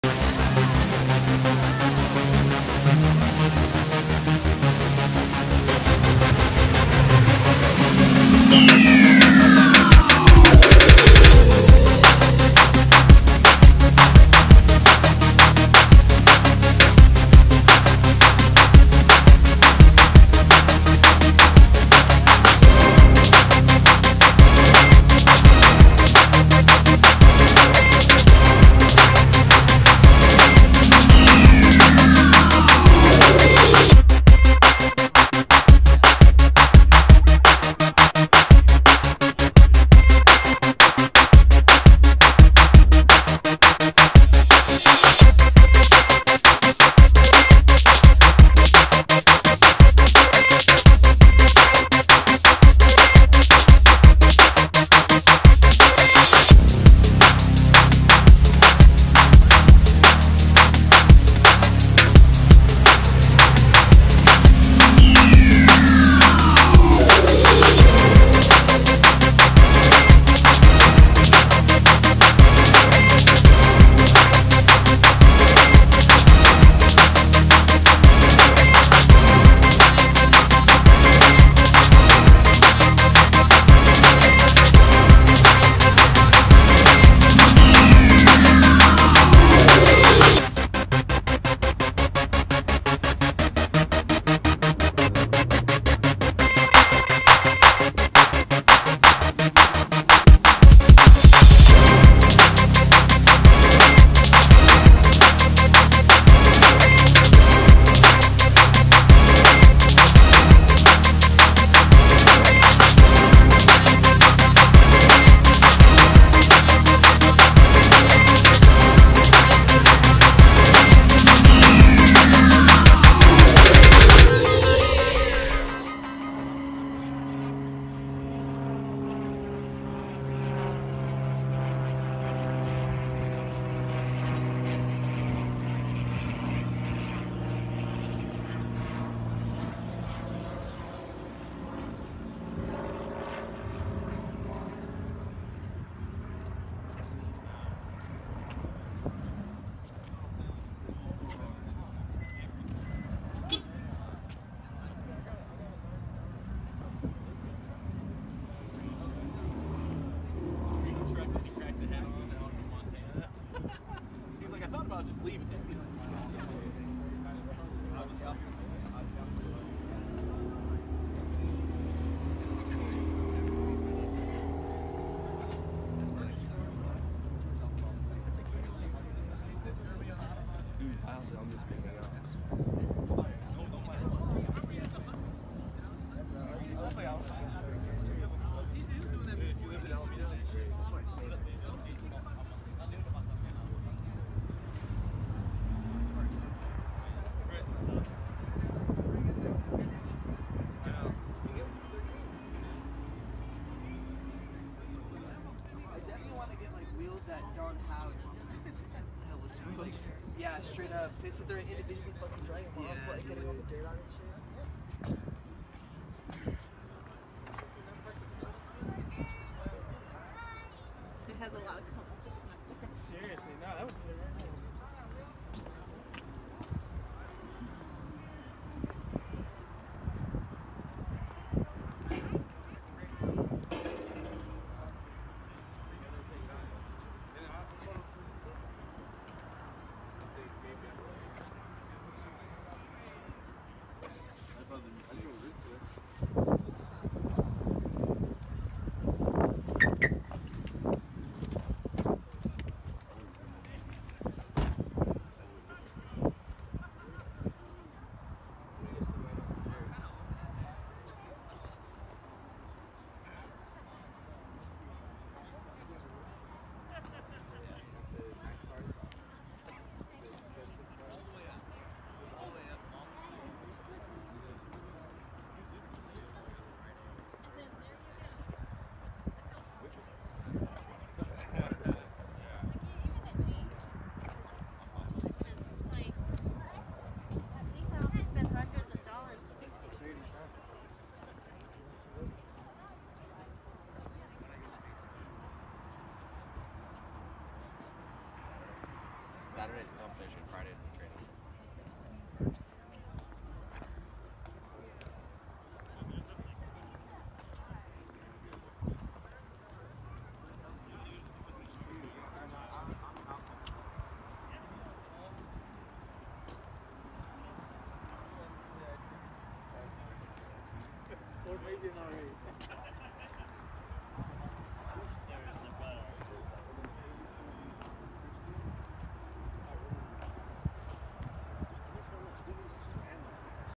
Cars and Coffee meet Concord CA 5/20/18
Hey what’s going on everybody? Here are some of the cars that were at the Cars and Coffee meet today 5/20/18.